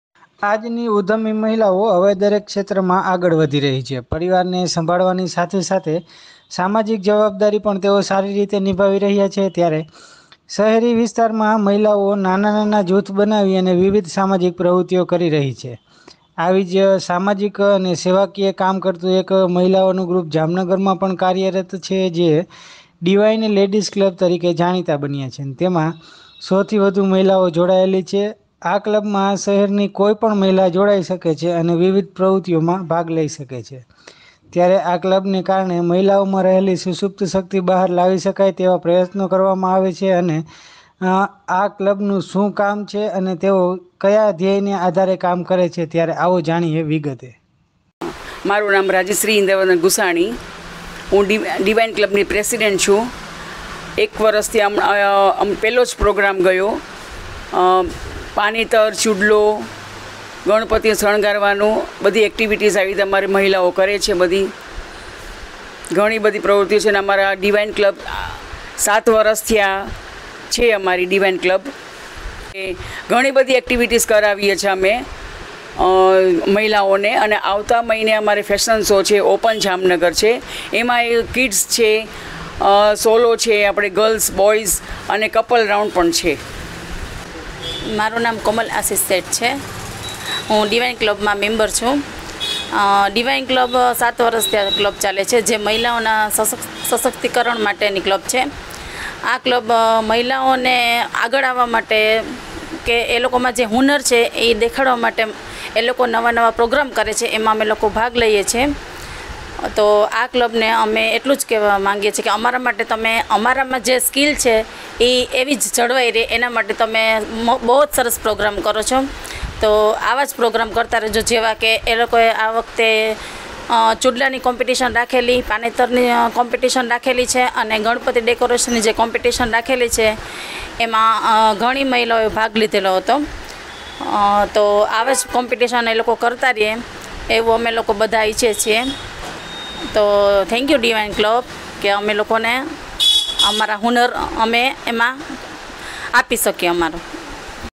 Ladies Club | સ્ત્રીઓની અનોખી પહેલ, ફેશન શો જેવા વિવિધ કાર્યક્રમો કરે છે મહિલાઓનું આ ક્લબ
Ladies Club | This women's club organizes various programs like unique initiatives of women, fashion shows

જામનગર: 21મી સદીની મહિલાઓ દરેક ક્ષેત્રમાં આગળ વધી રહી છે, પરિવારને સંભાળવાની સાથે સામાજિક જવાબદારી પણ સારી રીતે નિભાવી રહી છે. શહેરી વિસ્તારની મહિલાઓ તો નાના-નાના જૂથ બનાવીને વિવિધ સામાજિક સેવા કાર્યો પણ કરતી હોય છે. મહિલાઓની આતંરિક શક્તિઓને ઉજાગર કરવા ડિવાઇન લેડિઝ નામનું મહિલાઓનું ક્લબ જામનગરમાં કાર્યરત છે.